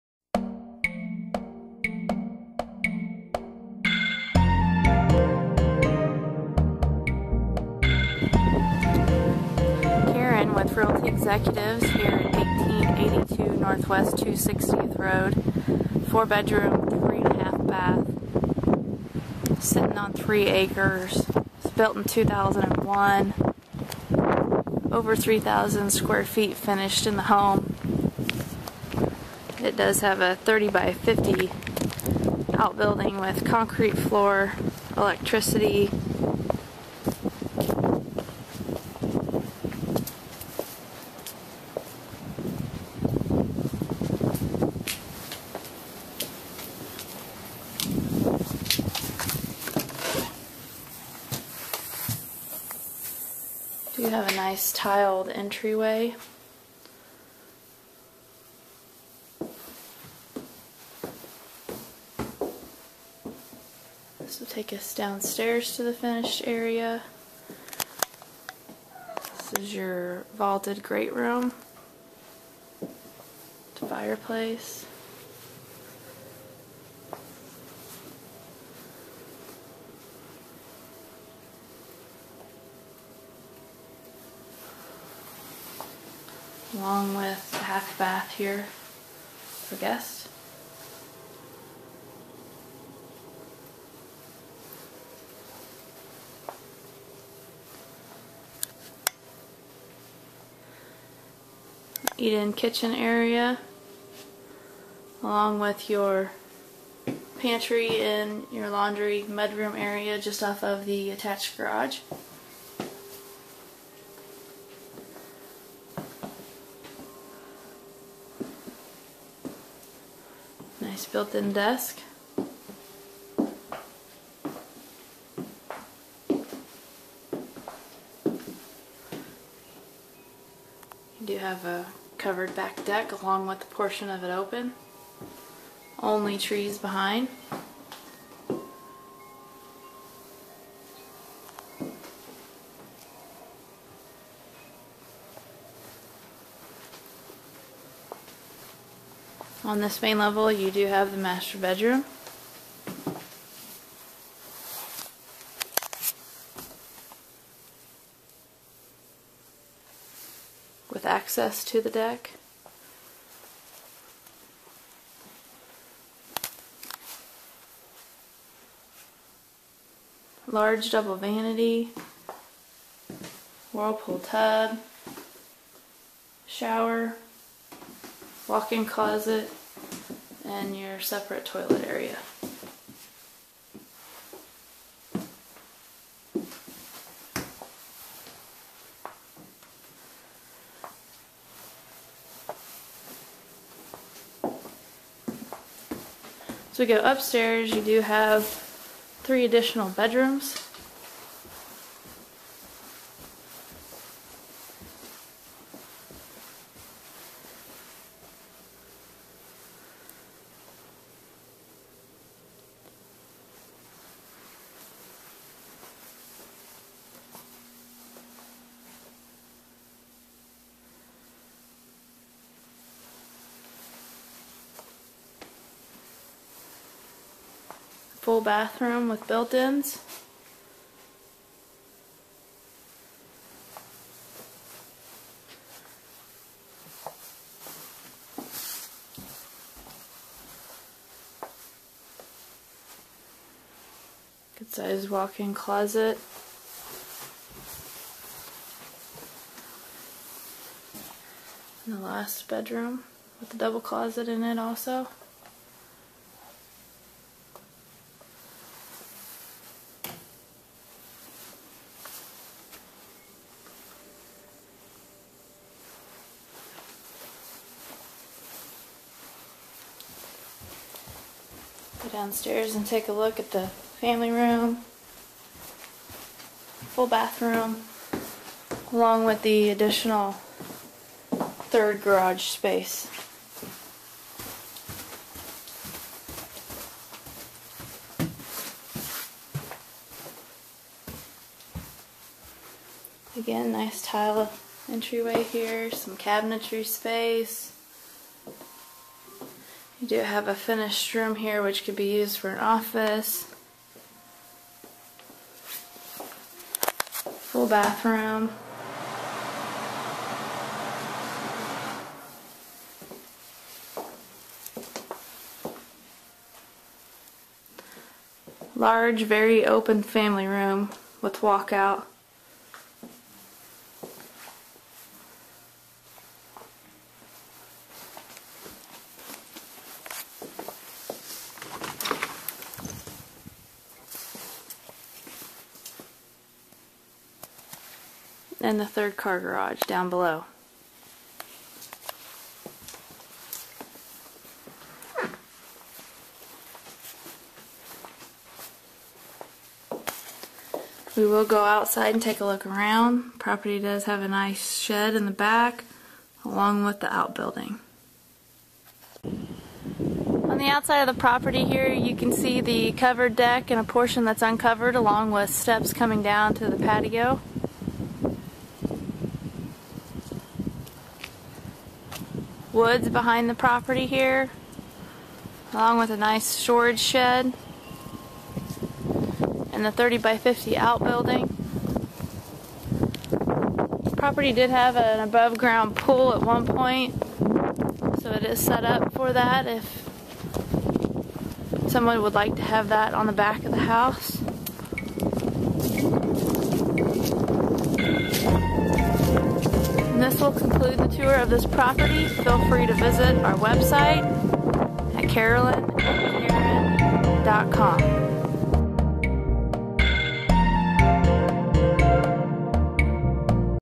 4 Bedroom Home on 3 Acres with 30x50 Outbuilding
Very nice and spacious 4 bedroom, 3 1/2 bath home - vaulted great room w/brick fireplace & balcony above, lots of windows, eat-in kitchen w/pantry, desk and eating bar, vaulted master w/walk-in closet, large double vanity, whirlpool and shower, 3 bedrooms on 2nd level with full bath, walk-out finished with full bath, family room and office, 30x50 outbuilding, concrete floor and electricity, 4 overhead doors, with lots of storage. 3rd car garage in walk-out w/wash tub and toilet, large deck off dining and master suite.